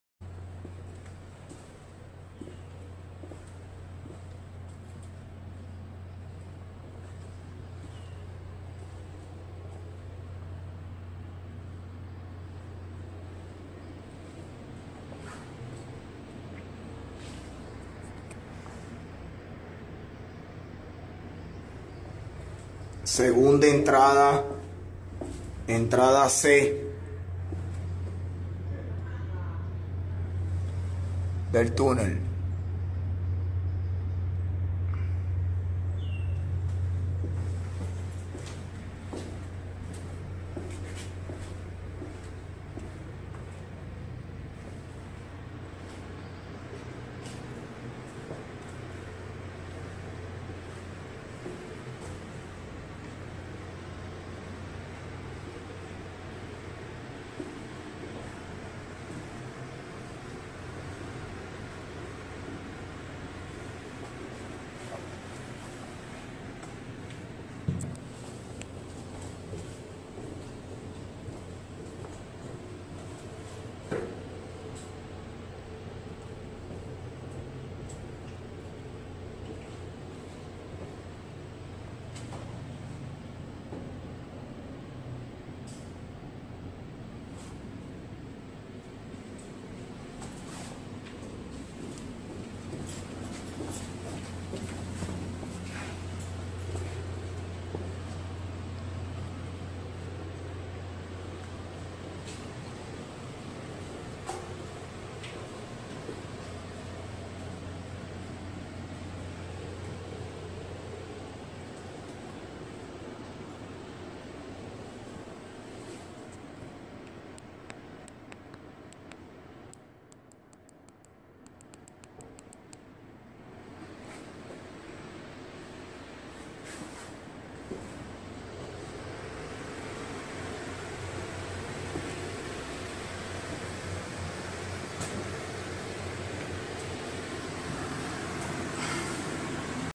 Alto Mando construyó túnel en Barquisimeto para ocultarse en un conflicto armado: Sujú
La abogada y defensora de DDHH Tamara Sujú aseveró este jueves que el alto mando militar construyó un túnel para esconderse en un posible conflicto bélico en Barquisimeto, estado Lara.

A través de Twitter, Sujú presentó un túnel ubicado en la 14 brigada de Infantería Mecanizada en Barquisimeto. "Alto mando construyó túneles en todo el país para ocultarse en un conflicto bélico, con asesoría cubana. A continuación, les presento uno de los túneles. Está ubicado en la 14 Brigada de Infantería Mecanizada en Barquisimeto, Edo Lara. Mide 157 mts de largo y 8 metros de ancho", escribió.

En el video, un hombre que no es identificado describe el túnel "segunda entrada, entrada C del túnel", a la vez que hace un recorrido por el mismo y muestra una habitación donde se ve una bandera de Venezuela y una sala de reuniones.

Video: Tamara Sujú.